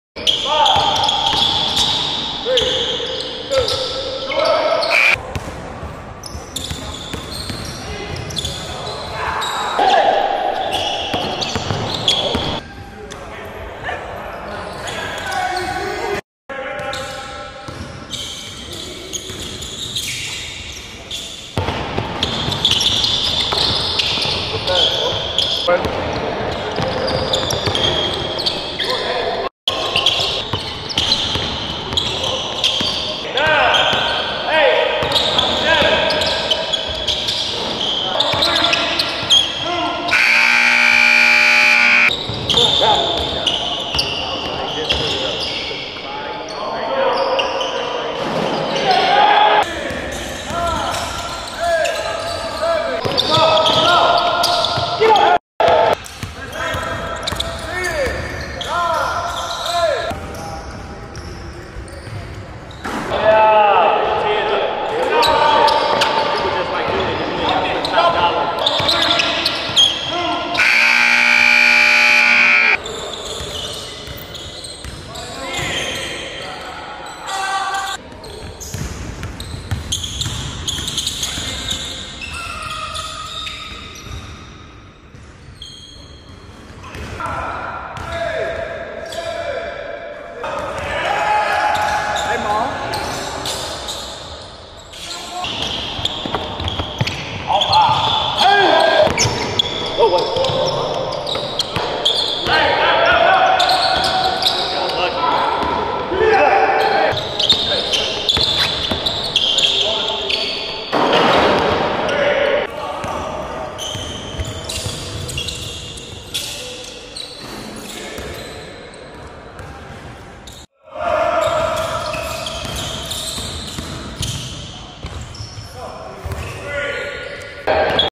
Darius Garland's hesi is lethal He was cooking at Jamal Richardson's runs
BASKETBALL GAMES WITH CRAZY GAMEPLAYS